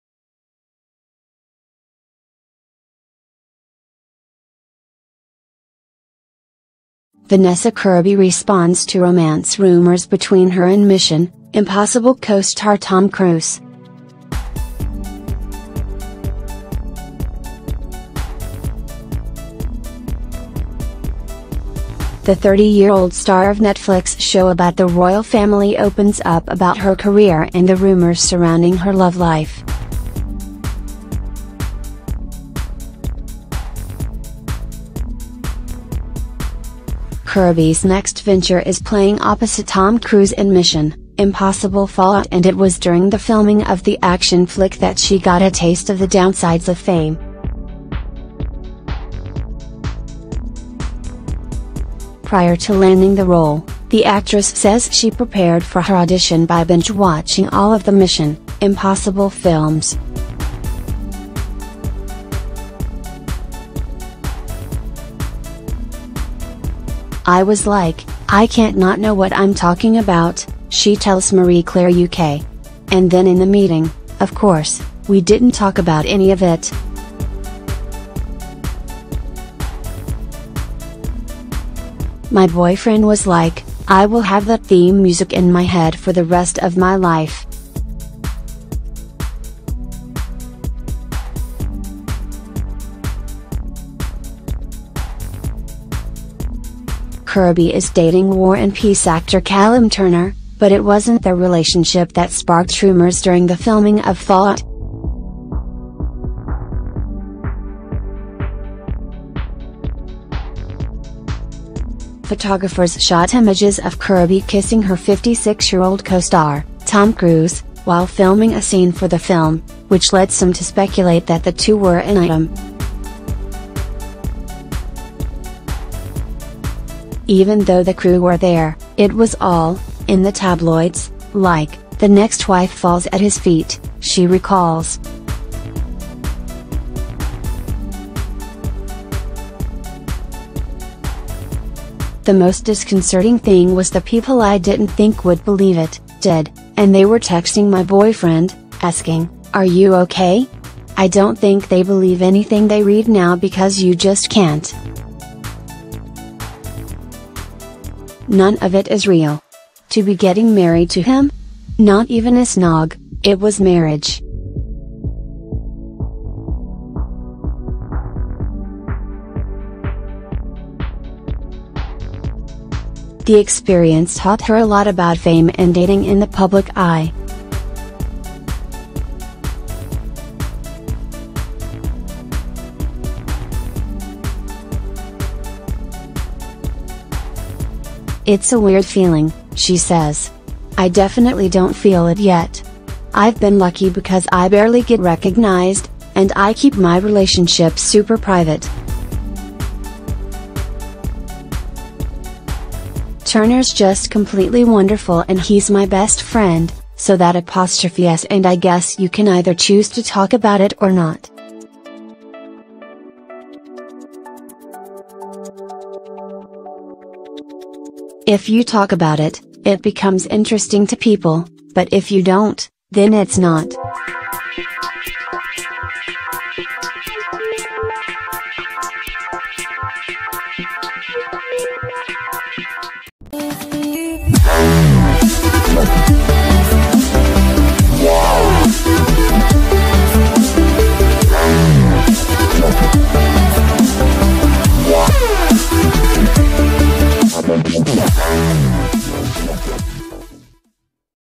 Vanessa Kirby Responds to Romance Rumors Between Her and ‘Mission: Impossible’ Co-Star Tom Cruise
Vanessa Kirby’s love life isn’t quite as scandalous as Princess Margaret’s on The Crown!